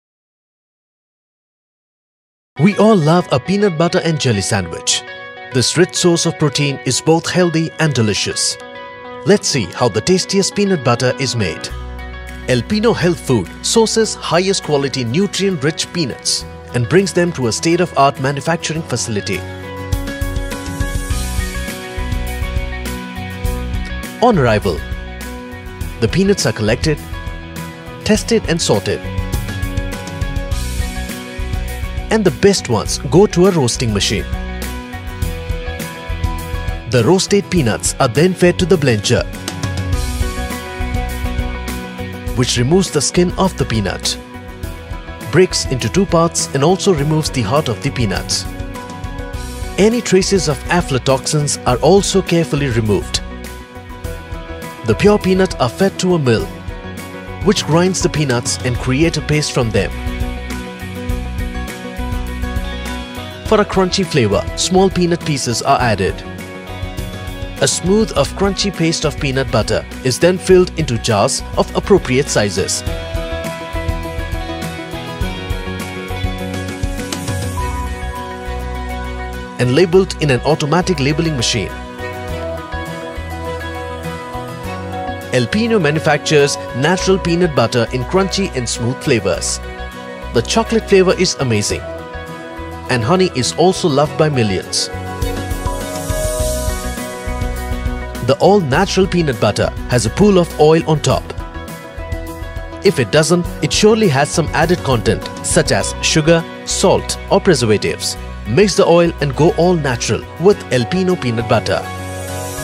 Alpino Organic Peanut Butter 400 G | Unsweetened | for buy click on the link in the description box
This is a Vegetarian product.
Made With Only Roasted Peanuts (100%). No Added Sugar / No Added Salt / No Hydrogenated Oils / 100% Non-GMO / Gluten Free.
10g Of Protein, 15g Of Healthy Fats, 0g Trans Fat & 0mg Cholesterol Per Serving Of 32g.
Oil Separation Is Natural Process, STIR WELL BEFORE USE Together With The Confidence Of Knowing Your Peanut Contains No Stabilizers Or Fillers.
Manufactured Under US FDA, APEDA, BRC, FSSC, HALAL, KOSHER, FSSAI, GMP, ISO 22000 Certified Facility, Under Strict Quality Control.

For buy alpino peanut butter click on the link-